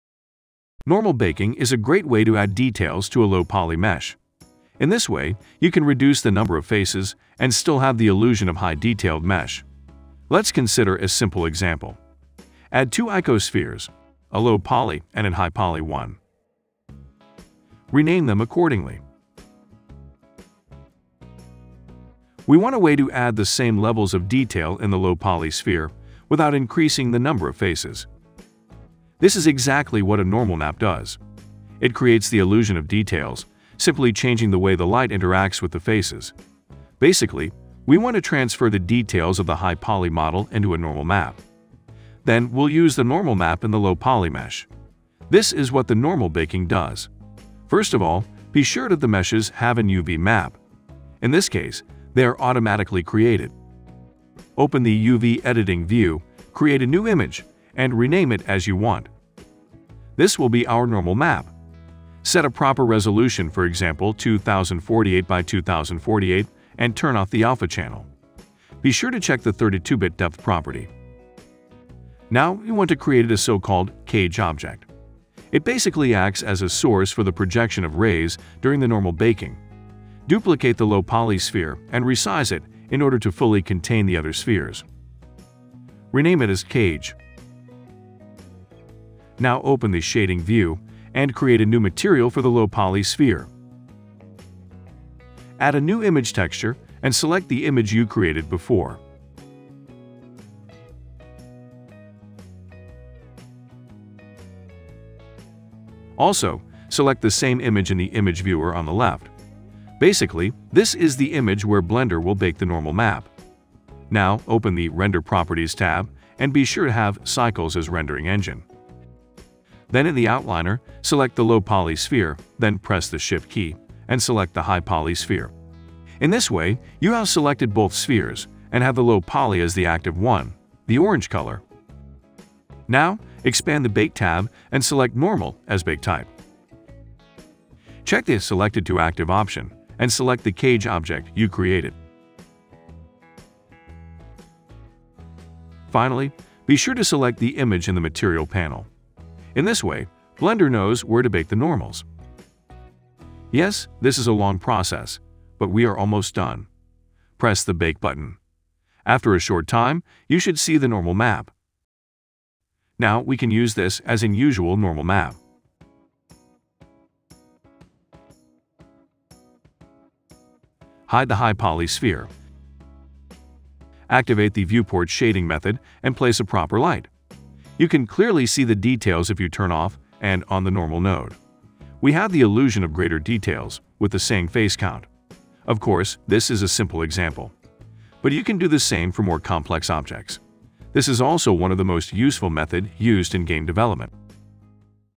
Normal map baking in Blender
Blender quick tips.
You can give the illusion of fine details to a low poly mesh, by baking a normal map from an hi-poly mesh of the same object.